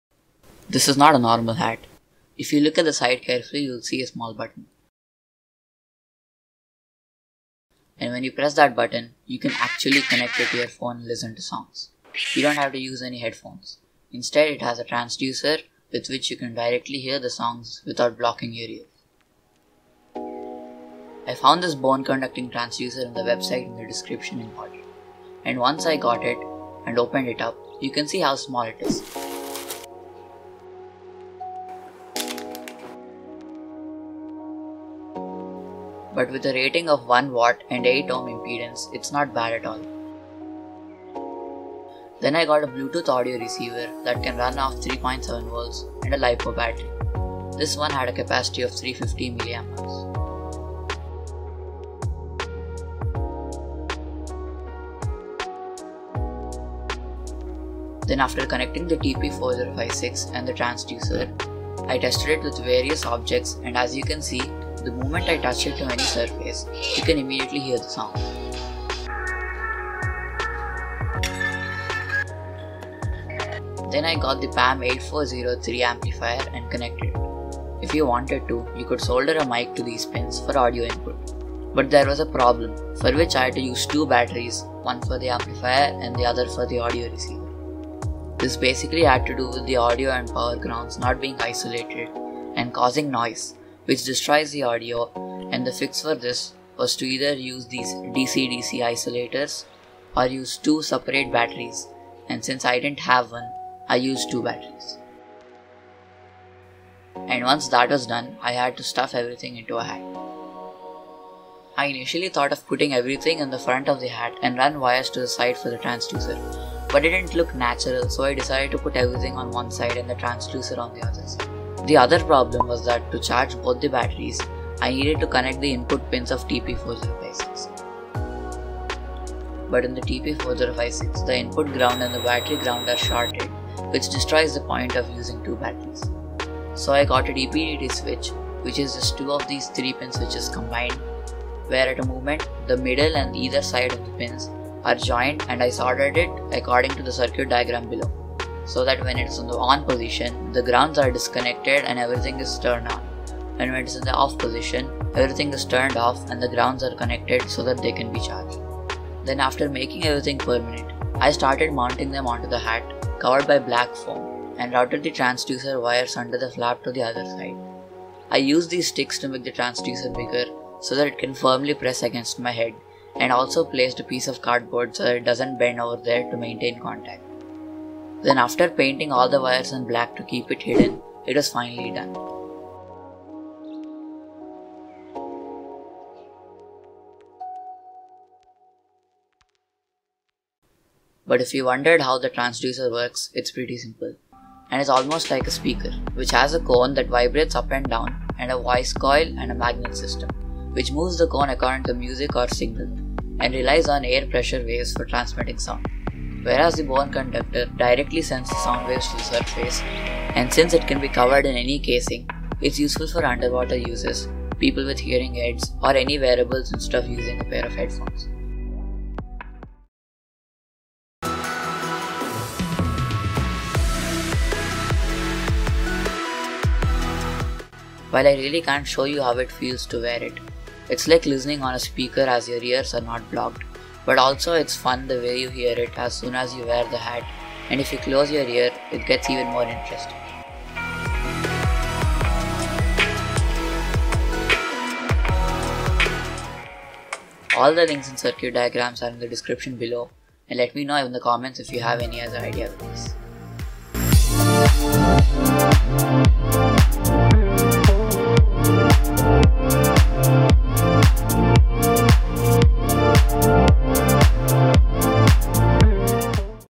Vibrating Hat Plays Songs | Bone Conduction Hat
Another Video Uploaded! Hope you enjoy the video!
---Info---
The Wiring is fairly simple similar to that of the Bluetooth speaker

With these Batteries it doesn't  really last very long But it also charges Very quickly so it can be upgraded.
This projects is more of a gimmick or a fun thing to make.
I recommend getting a big Hat that covers your head till your ear and a stiff one if possible.
All the Links are given below!
---Info---

Components
Bluetooth Audio Receiver

PAM8403 3W amplifier

Bone Conductor Transducer (Sold out , Available in DIGIKEY)
Components